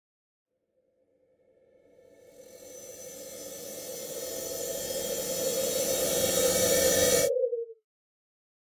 MSM: TAQ | Avarian Casino | Šroombal
Just to be clear here, this is not the quad. He’s actually a double-elemental I forgot to release!
Silly me!
**SPECIES:** Šroombal
**ELEMENTS:** Snot/Lumi
**CLASS:** Avarian
**GENDER:** We don’t know, since they’re a mushroom.
**SOUND:** Any cymbal soundfont (changes on each island)
**HOW THEY PLAY:** Their cap lights up.
**WEAPON:** Slime and the _hypnotic_ effects it can give you…
**BIO:** Šroombal never used to be an Avarian. They were a type of mushroom related to the Glowshrooms. But they have powers that allow them to travel anywhere they want… so they went here, and needless to say, they already enjoy it in the Avarian Casino. Their slime can have _hypnotic_ effects… but that’s only if you don’t cook it! That’s right! Avarians eat goo as a delicacy! Have you tried it yet?